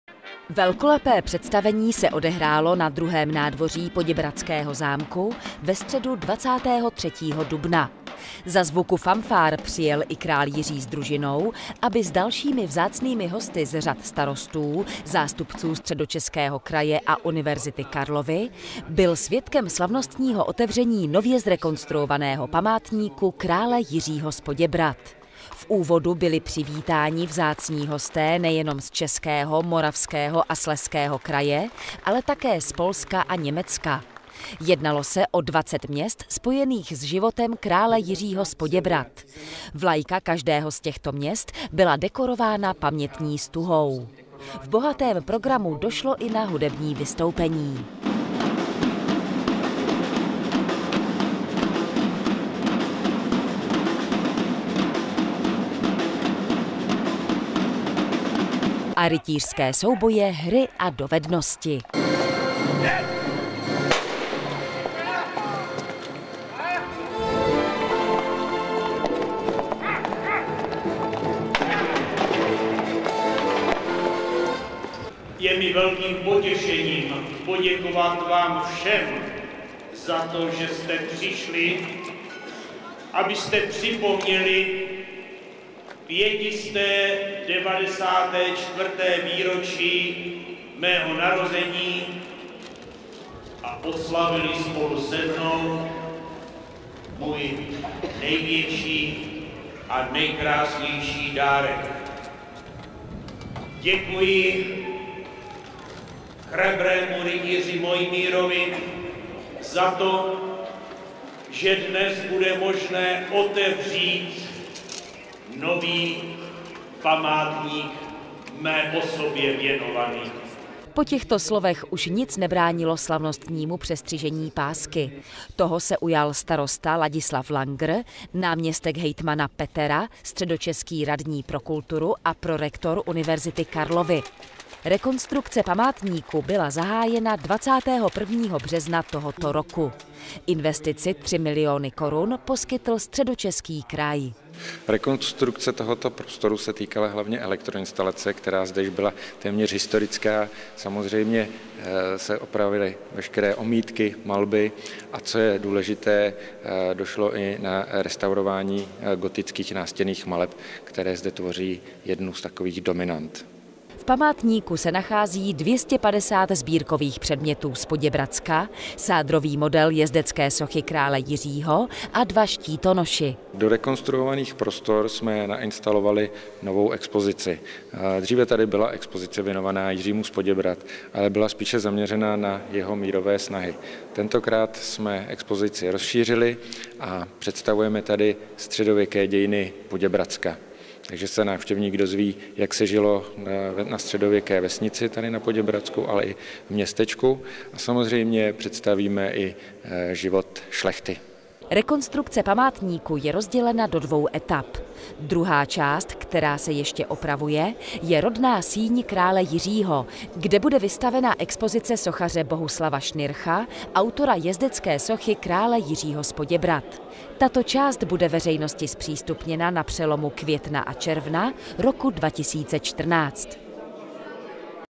PODĚBRADY - Slavnostní otevření Památníku krále Jiřího z Poděbrad
Velkolepé představení se odehrálo na II. nádvoří poděbradského zámku ve středu 23. dubna. Za zvuku famfár přijel i král Jiří s družinou, aby s dalšími vzácnými hosty z řad starostů, zástupců Středočeského kraje a Univerzity Karlovy, byl svědkem slavnostního otevření nově zrekonstruovaného Památníku krále Jiřího z Poděbrad.

V úvodu byli přivítáni vzácní hosté nejenom z českého, moravského a slezského kraje, ale také z Polska a Německa. Jednalo se o 20 měst spojených s životem krále Jiřího z Poděbrad. Vlajka každého z těchto měst byla dekorována pamětní stuhou. V bohatém programu došlo i na hudební vystoupení a rytířské souboje, hry a dovednosti. Po projevu krále Jiřího už nic nebránilo slavnostnímu přestřižení pásky. Toho se ujal starosta Ladislav Langr, náměstek hejtmana Miloš Petera, středočeský radní pro kulturu Zdeněk Štefek a prorektor Univerzity Karlovy Martin Kovář. Rekonstrukce památníku byla zahájena 21. března tohoto roku. Investici 3 milióny korun poskytl Středočeský kraj. V Památníku se nachází 250 sbírkových předmětů z Poděbradska, sádrový model jezdecké sochy krále Jiřího a dva štítonoši. Rekonstrukce památníku je rozdělena do dvou etap. Druhá část, která se ještě opravuje, je rodná síň krále Jiřího, kde bude vystavena expozice sochaře Bohuslava Schnircha, autora jezdecké sochy krále Jiřího z Poděbrad. Tato část bude veřejnosti zpřístupněna na přelomu května a června roku 2014.

Redaktor: Zuzana Jáklová, kamera a střih: Luděk Vondrouš, komentář četla: Nikola Birklenová